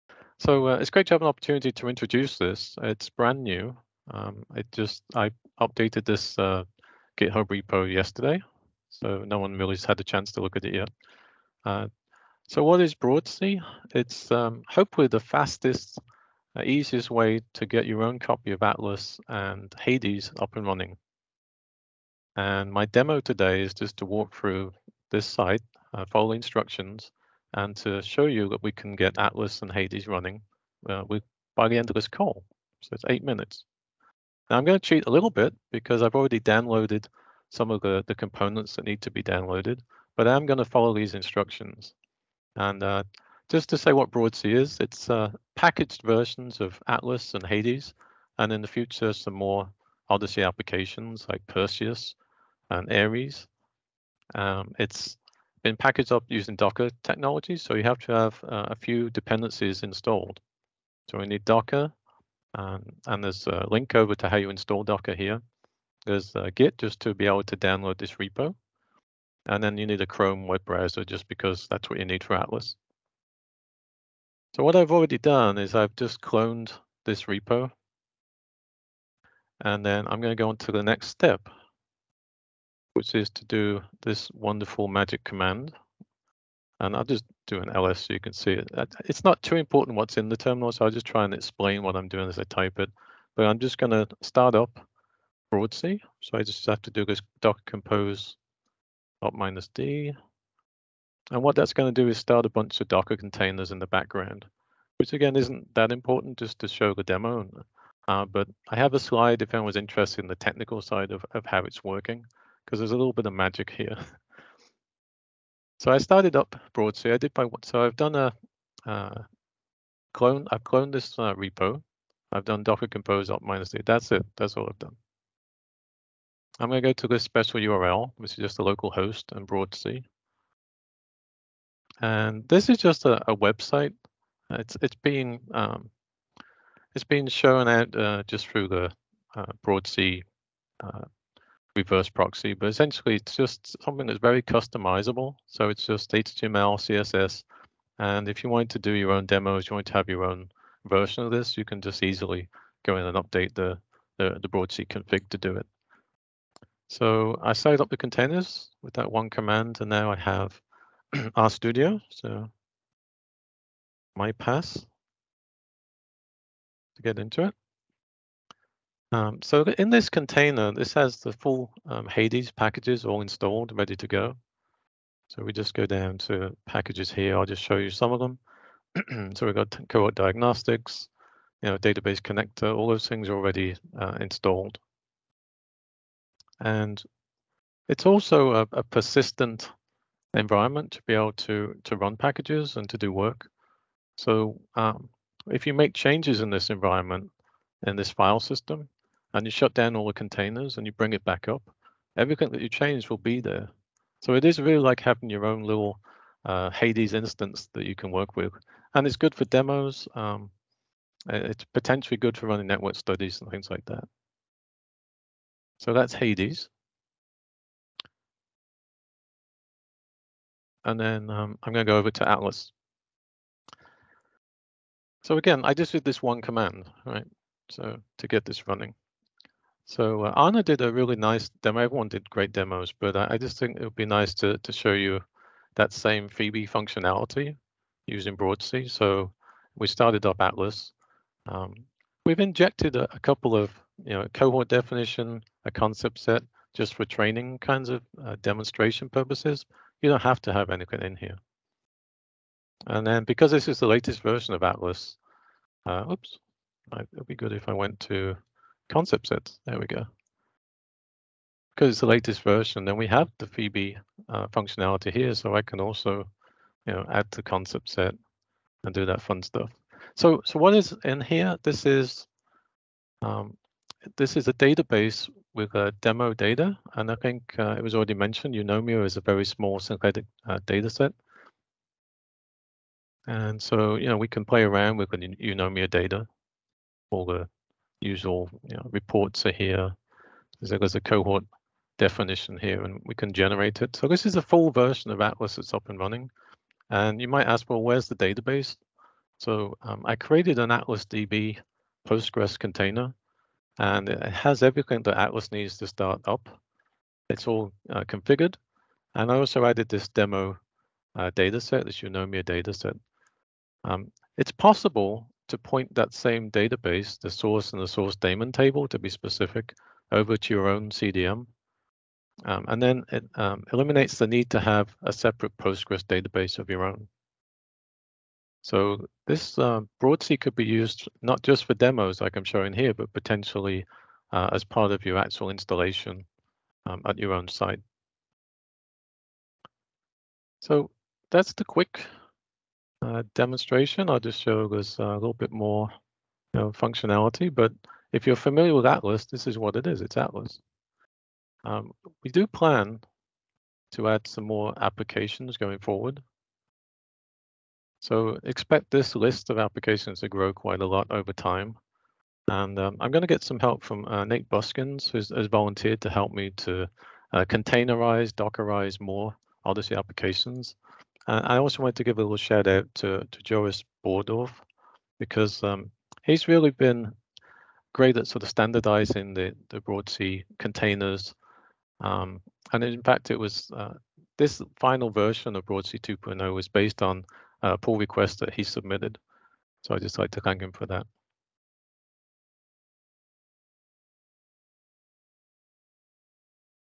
Tutorial: Broadsea (Lee Evans)
During the Nov. 22 "10 Minute Tutorials" Community Call, Lee Evans (Founder, LTS Computing LLC) presented Broadsea, which runs the core OHDSI technology stack (The Atlas web application, and the Hades R library within RStudio Server), using cross-platform Docker container technology.

Recordings of all past Community Call presentations, as well as links shared during all updates, are posted